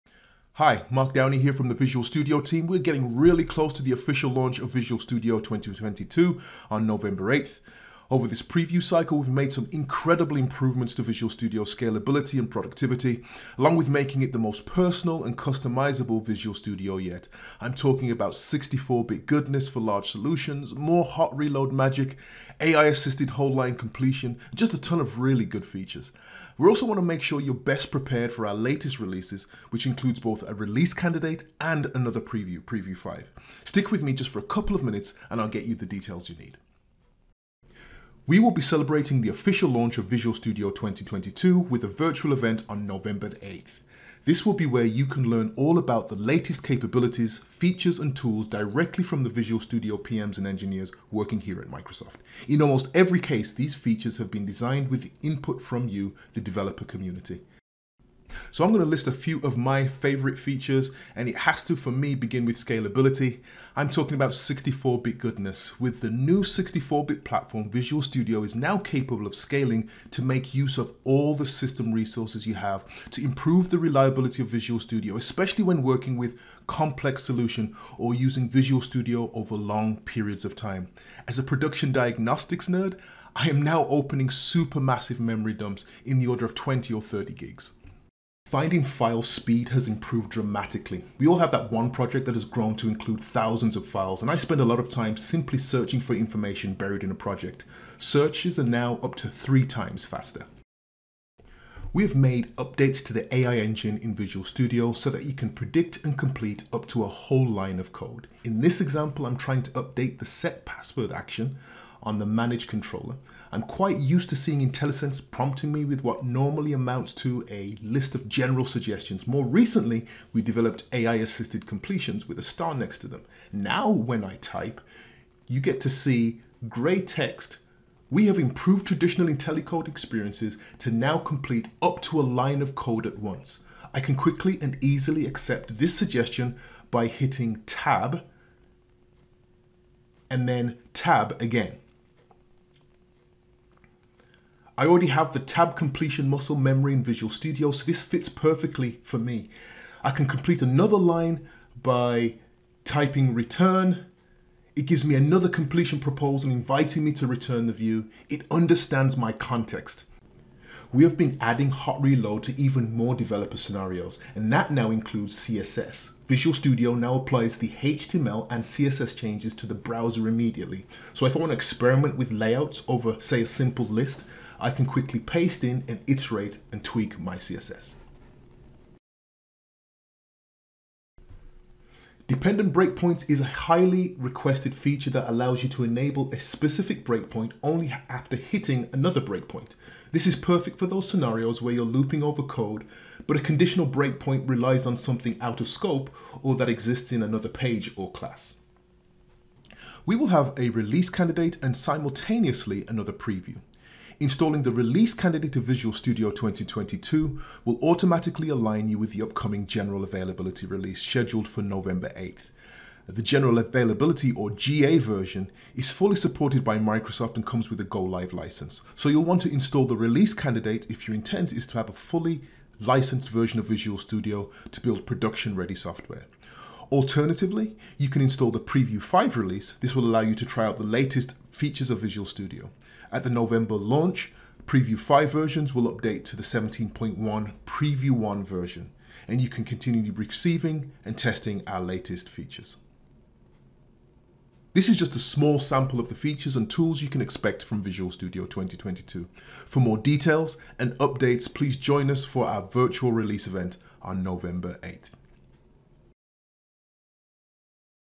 Visual Studio 2022 - Launching 2021-11-08
We're excited to announce the upcoming release of Visual Studio 2022 on November 8 and the immediate availability of the Visual Studio 2022 Release Candidate (RC) and Preview 5. We invite you to explore the latest capabilities of Visual Studio 2022 at our virtual launch event on November 8

0:00 Intro
0:39 Launch Event
1:31 Find in files
1:46 IntelliCode whole line completion
2:41 Hot Reload
3:03 Dependent breakpoints
3:25 Preview vs Release versions